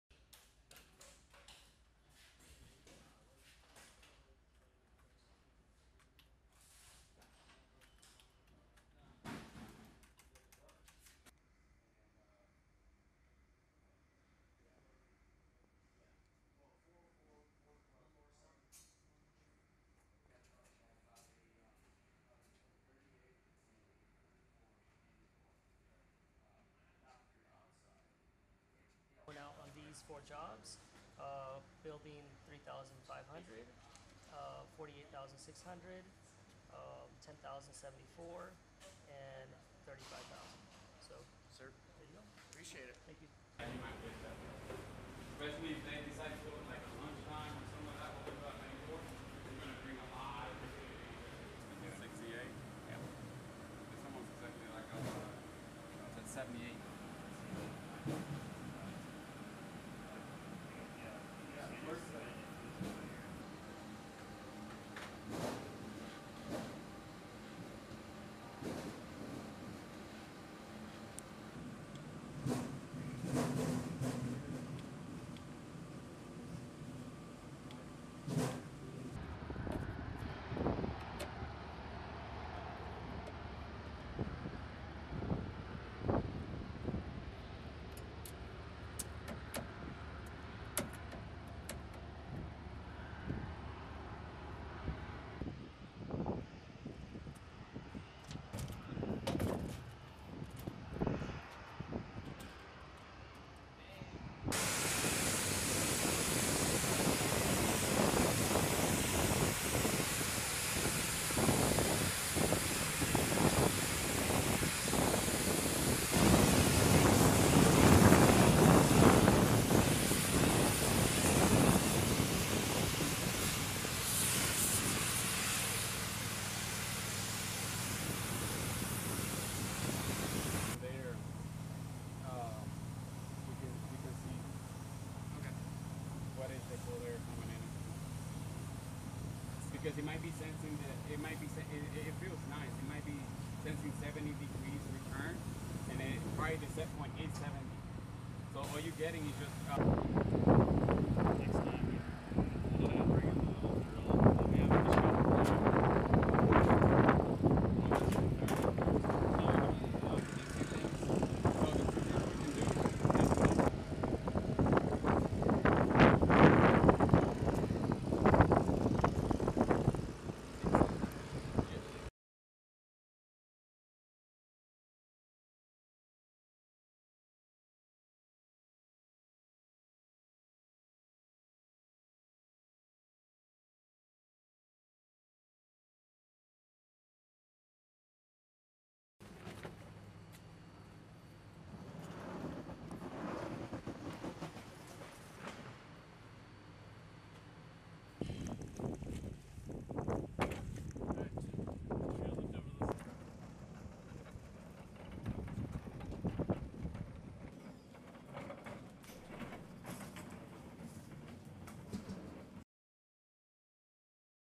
AUAB HVAC B-Roll
AL UDEID AIR BASE, QATAR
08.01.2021

Airmen assigned to the 379th Expeditionary Civil Engineer Squadron conduct HVAC operations on Al Udeid Air Base, Qatar.

Film Credits: Video by Staff Sgt. Kylee Gardner
379th Air Expeditionary Wing Public Affairs

Airmen
Deployed
Working
Civil Engineer
CE
HVAC